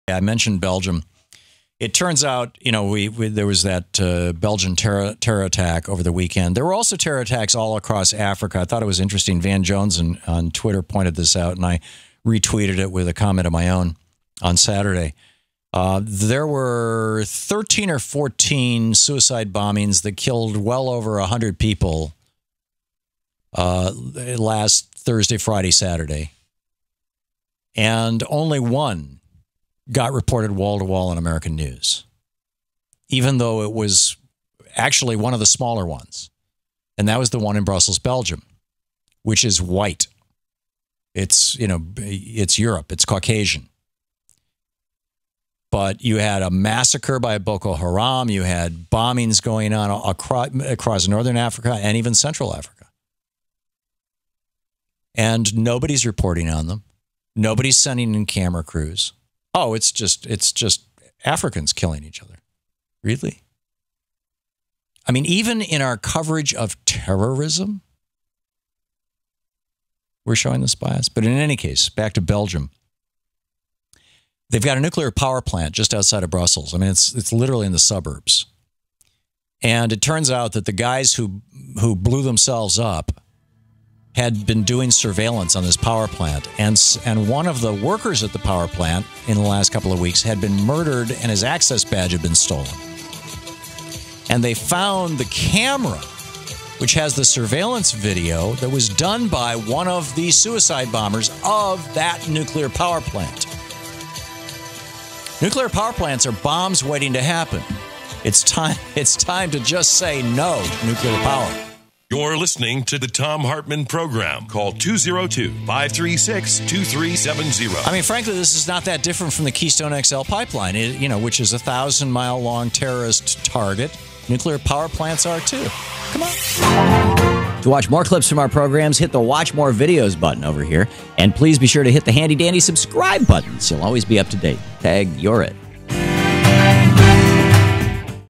The Case for Ending Nuclear Power
Thom Hartmann says terrorists have and continue to want to target nuclear power facilities to cause mass casualties.